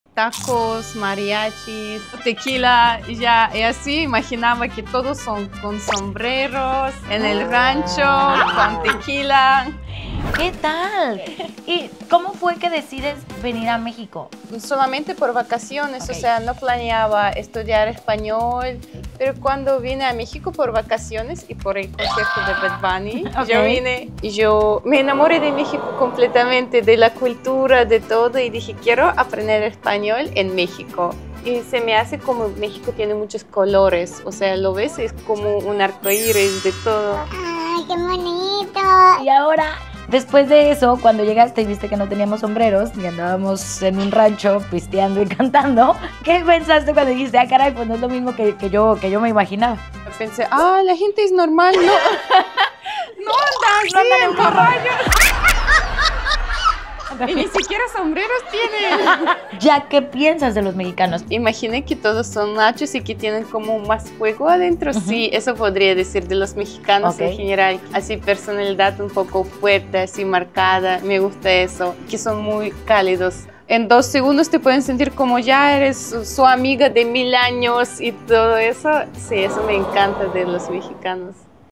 ¿Qué piensan las rusas de los mexicanos?
En este video, exploramos una fascinante perspectiva: ¿qué piensan las mujeres rusas sobre los mexicanos?

BADABUN NETWORK ©